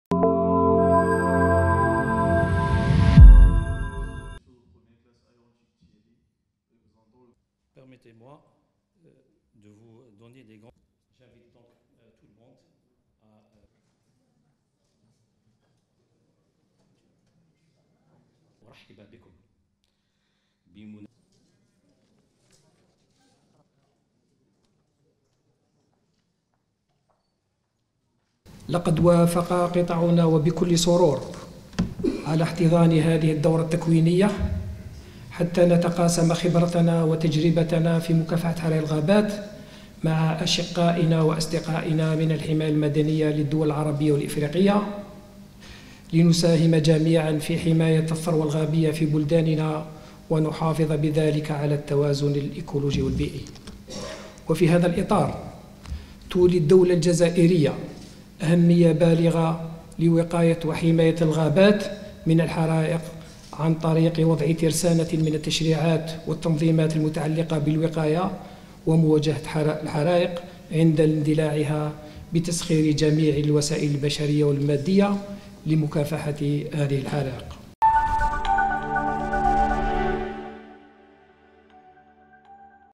الحماية المدنية: إفتتاح دورة تكوينية في مكافحة الحرائق لمتربصي دول عربية وإفريقية
الجزائر(وأج)- نظمت المديرية العامة للحماية المدنية دورة تكوينية في مجال قيادة عمليات مكافحة حرائق الغابات، لفائدة متربصين من 14 دولة عربية وإفريقية، وذلك على مستوى مستوى الوحدة الوطنية للتدريب والتدخل بالدار البيضاء على ان تختتم هذه الدورة في 28 جويلية الجاري.